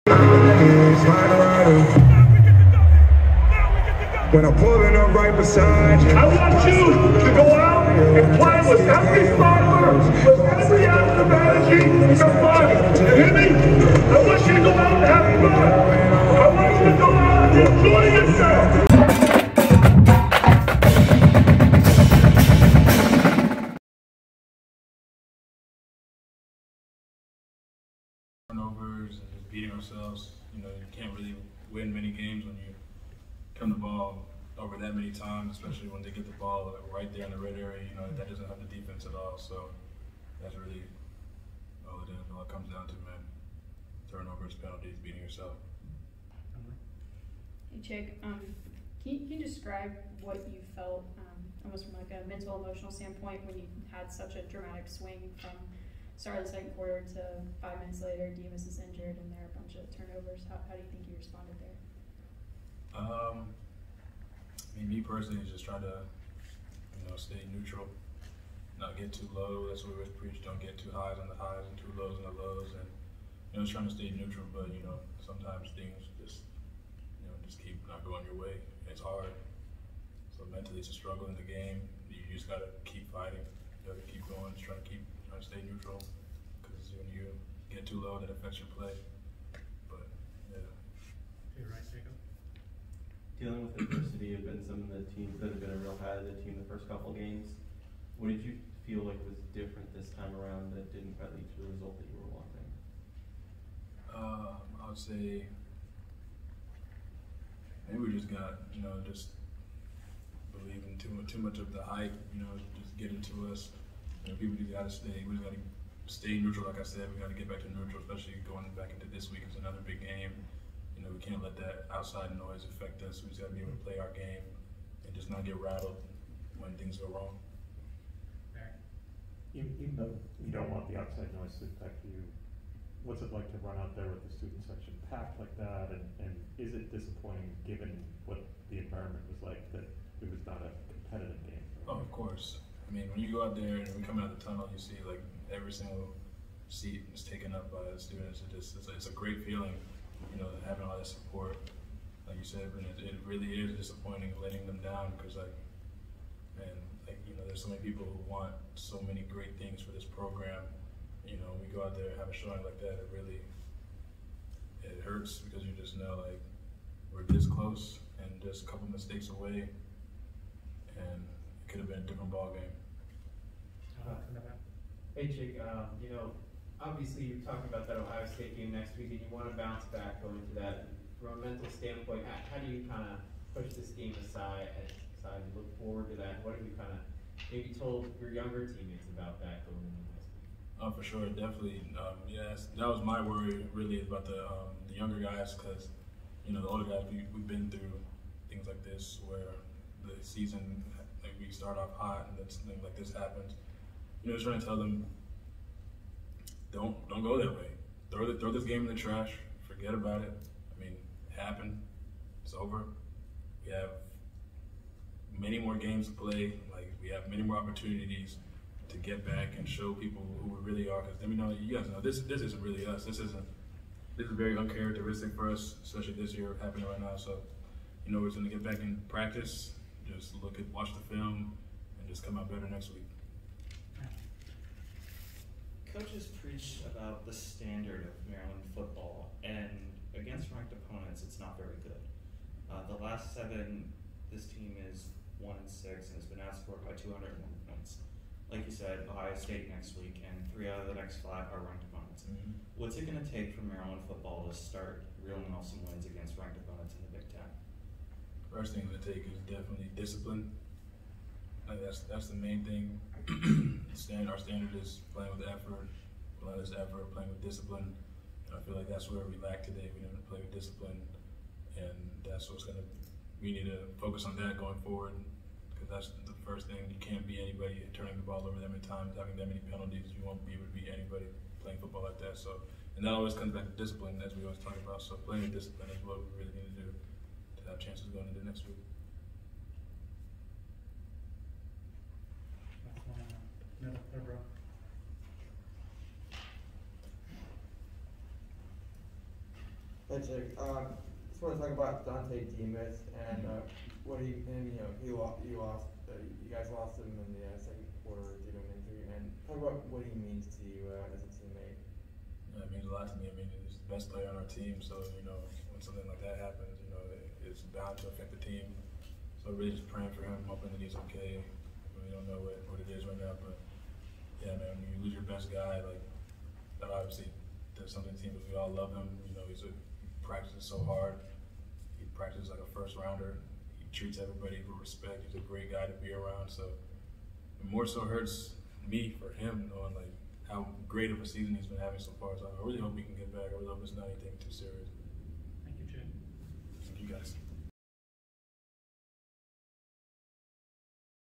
Chig Okonkwo Postgame Iowa - Maryland Football 2021
Maryland loses at home to Iowa 51-14.